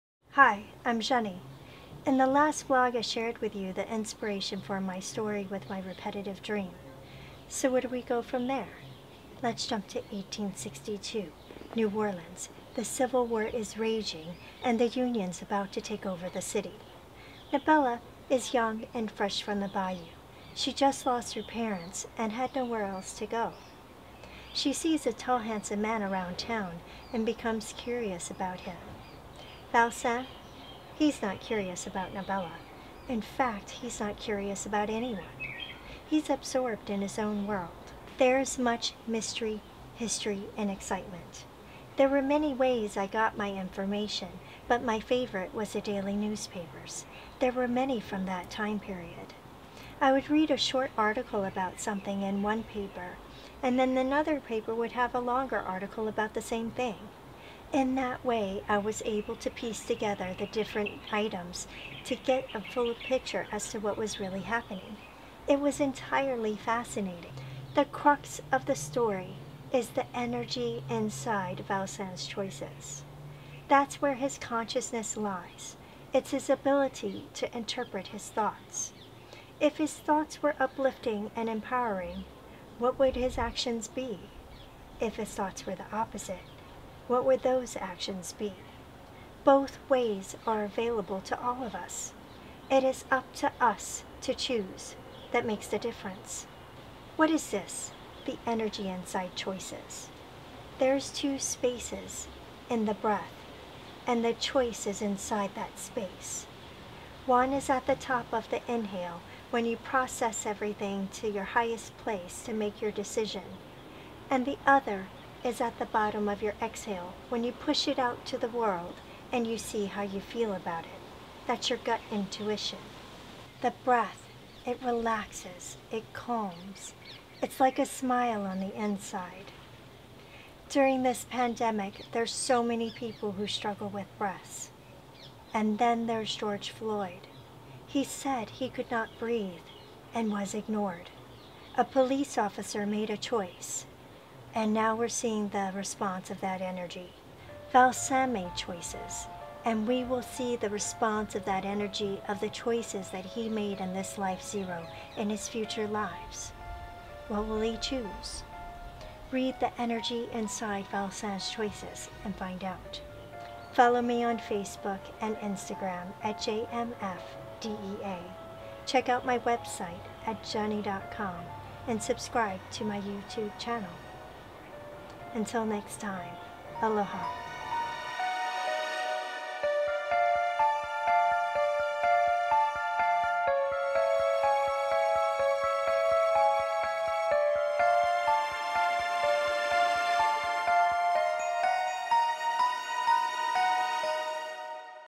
The Energy Inside Valsin's Choices - Life Zero
We jump into the Civil War with this part historical novel to get a sense of the main characters.
But what is the energy inside choices? The spirit is the breath and the breath the spirit. Think about it while you watch this video. Aloha